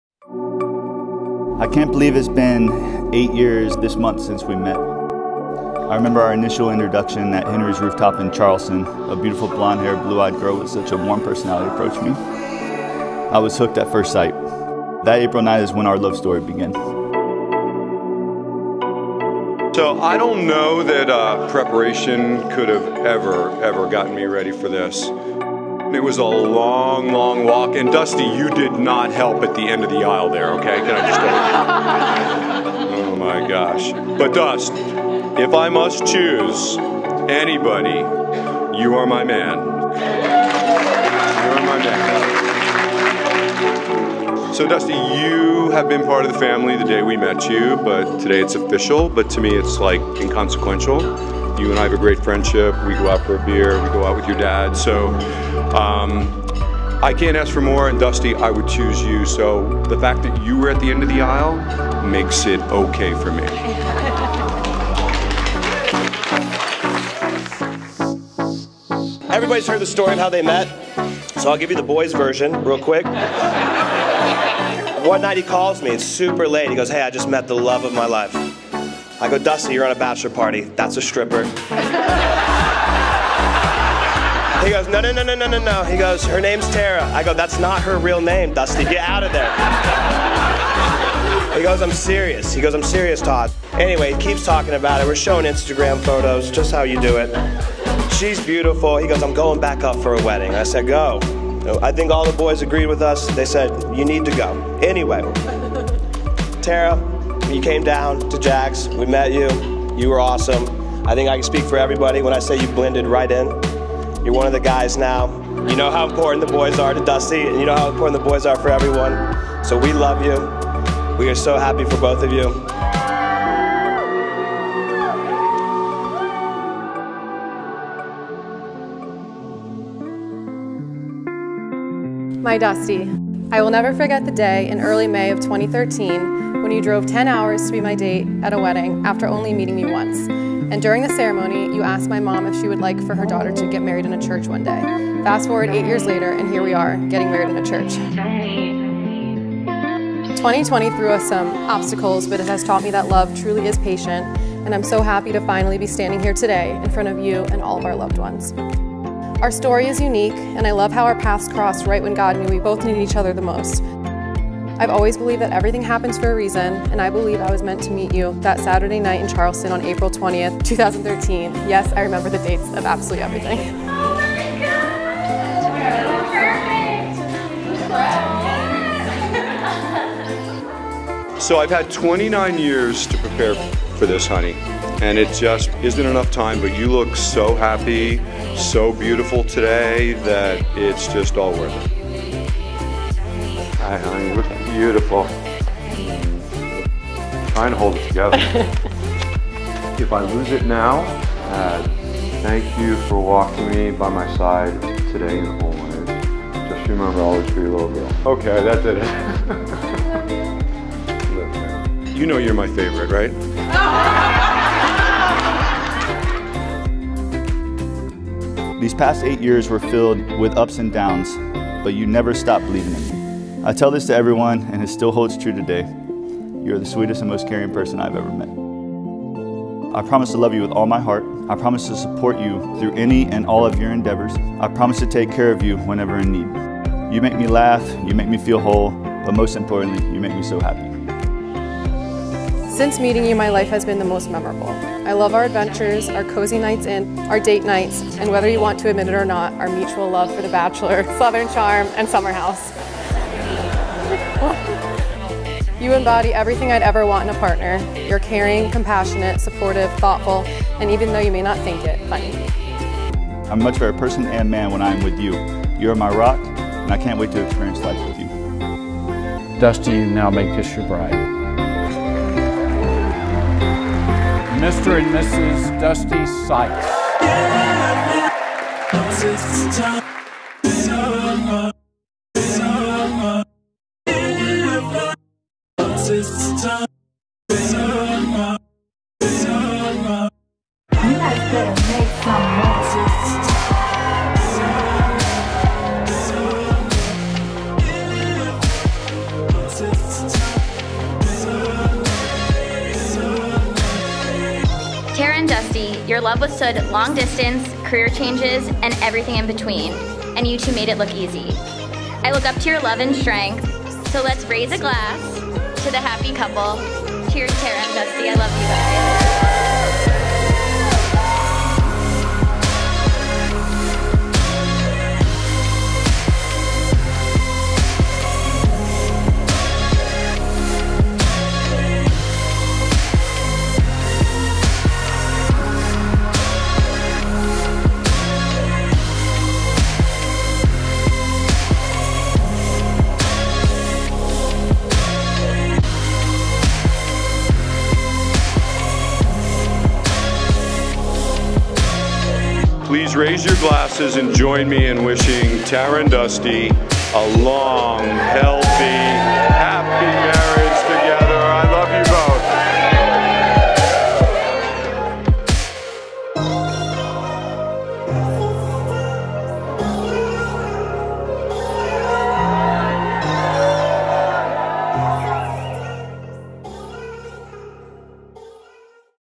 Amelia Island Wedding Video
Tara + Dusty //

Ceremony: Amelia Plantation Chapel
Reception: Amelia Island Club
Coordinator: Dairing Day Of
Hair/Makeup: Studio Bride
Florals: Island Flowers and Garden
Entertainment: The Leonard Brothers
Photography: Ashley Steeby
Videography: Ricker Films
Rentals: All About Events
Cake: Bliss Bakery
Photo Booth: Chic Booth
Dress: Pronovias